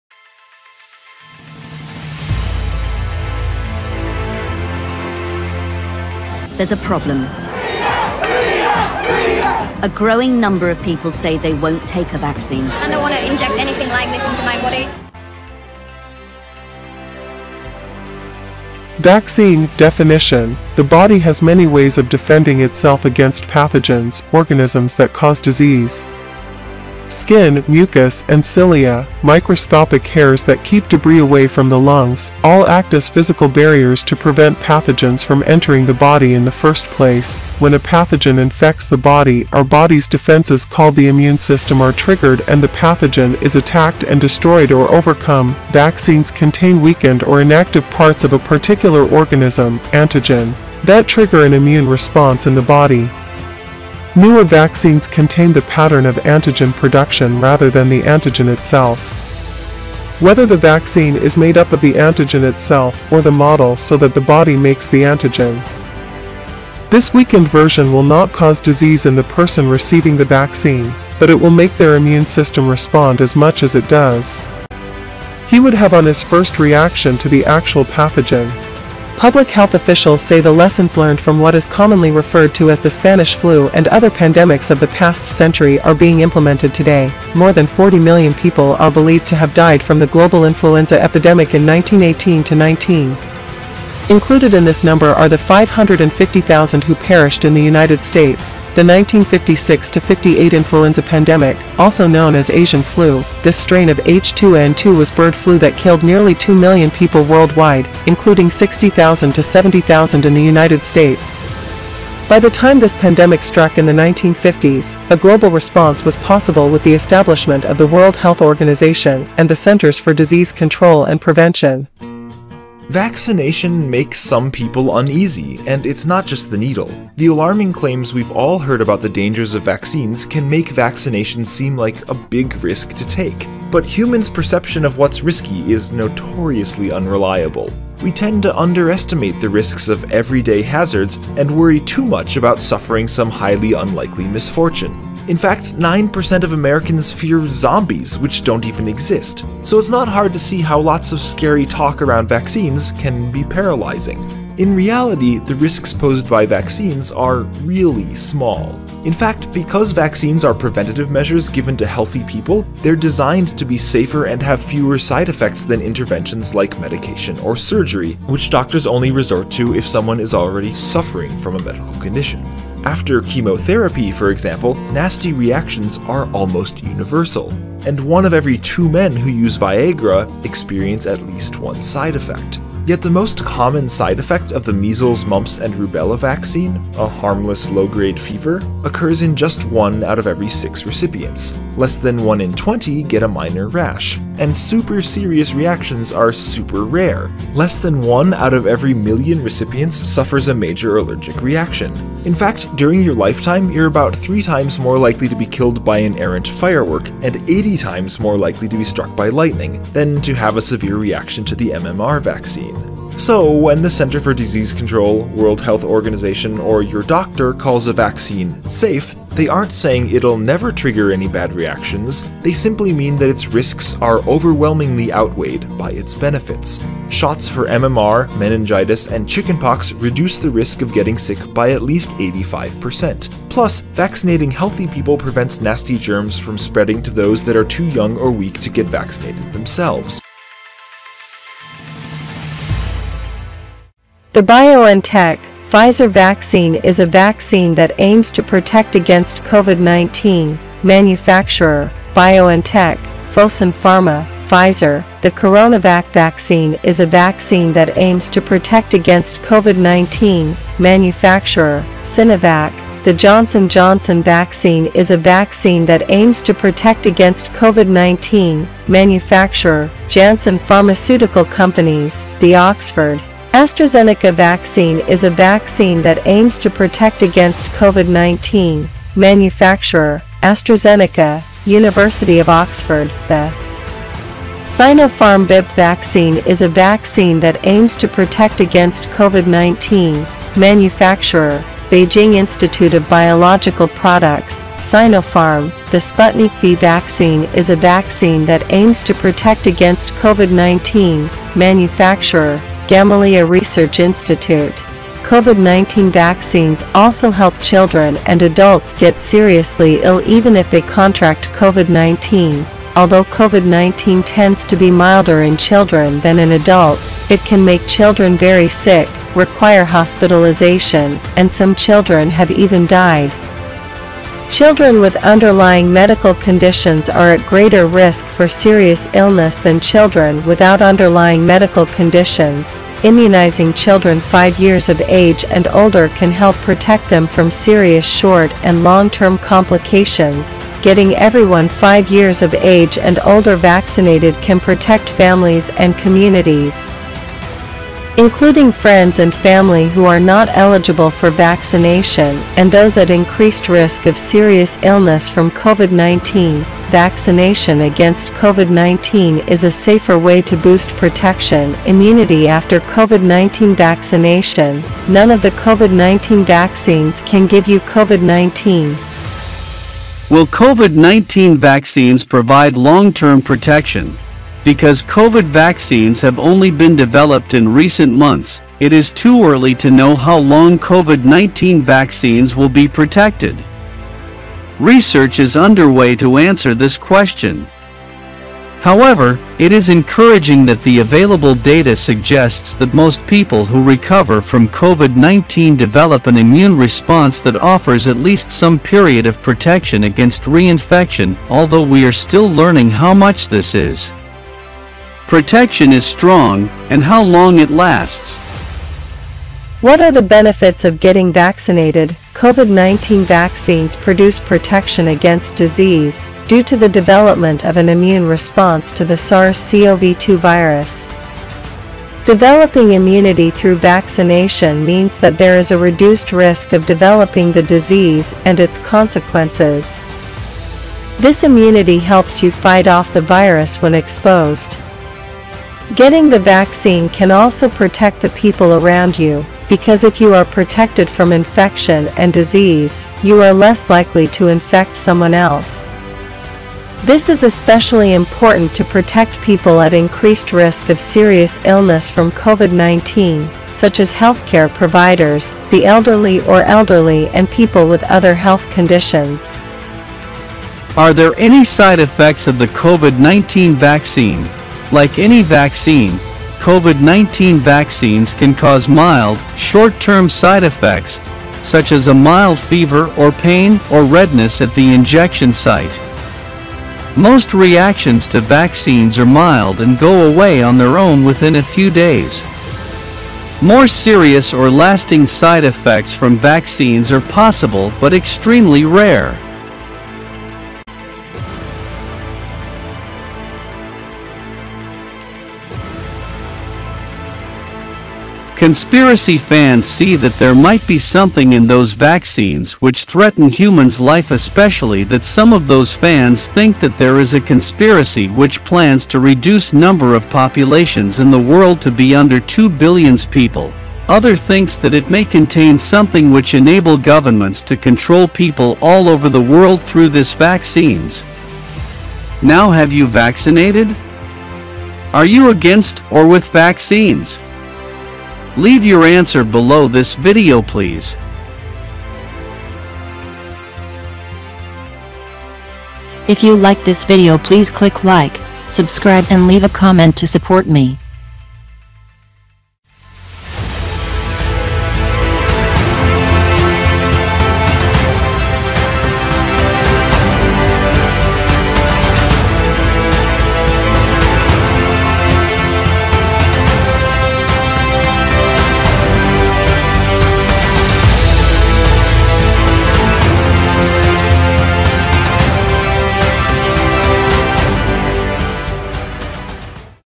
Vaccine. Have You Vaccinated? Are you with or against Vaccine.
Have you vaccinated? Are you against or with vaccine? what is the vaccine ? do you pros and cons of vaccines?
In this video I may add some information to your knowledge about vaccines.